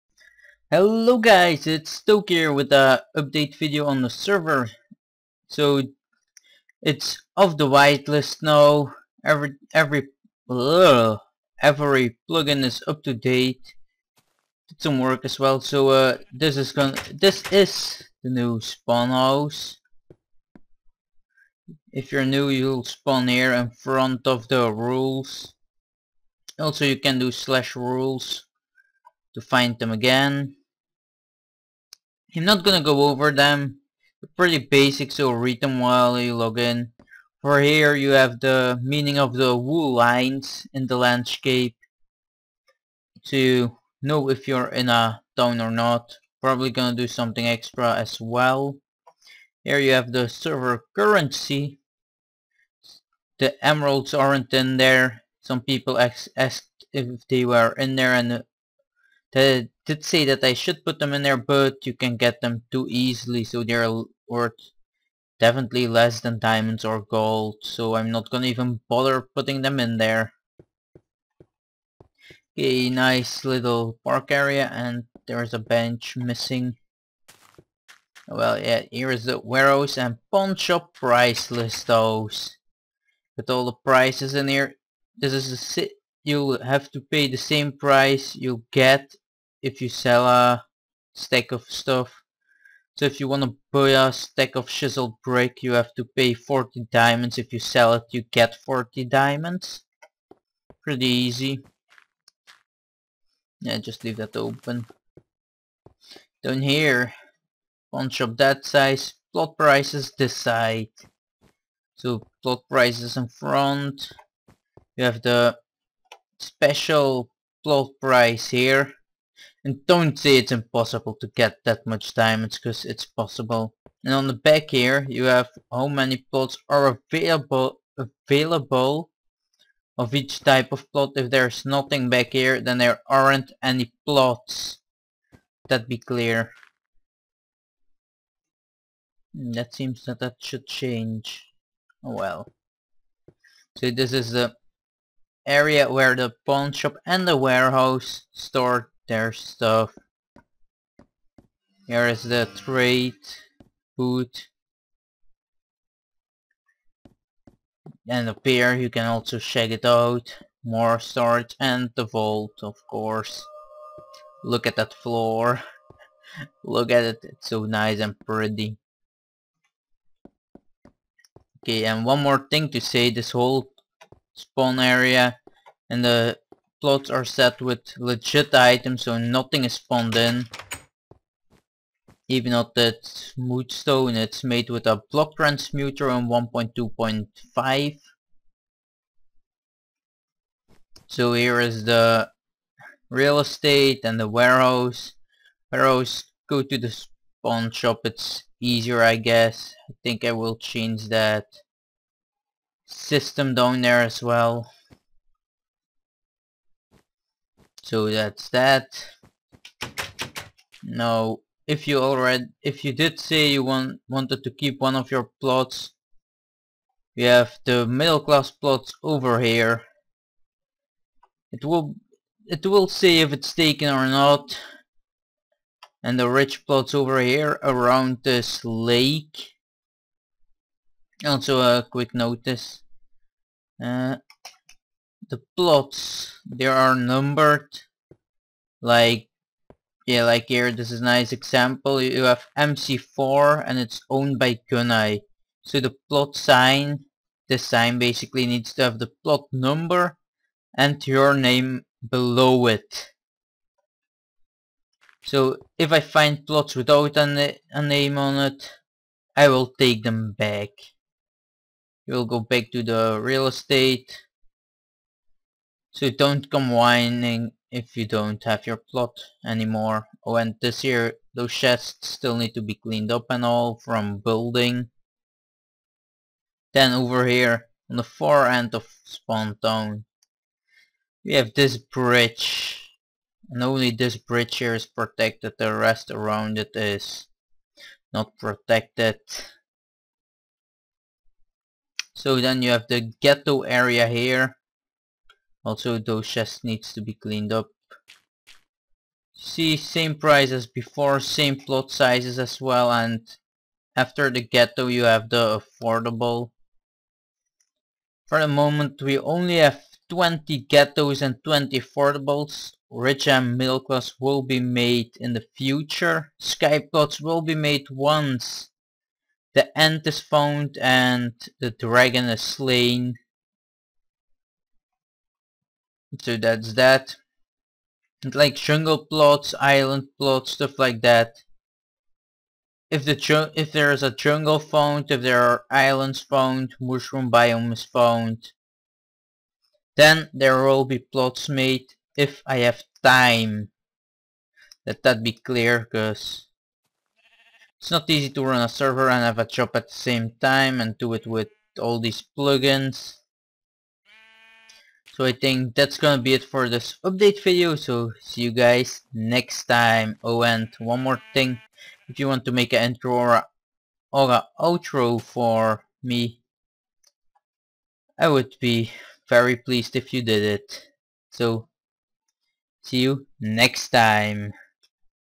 server to 1.3.1 all clear
ok guys the server is of the whitelist now
so com on and enjoy yourself whit the game

come on and anjoy the game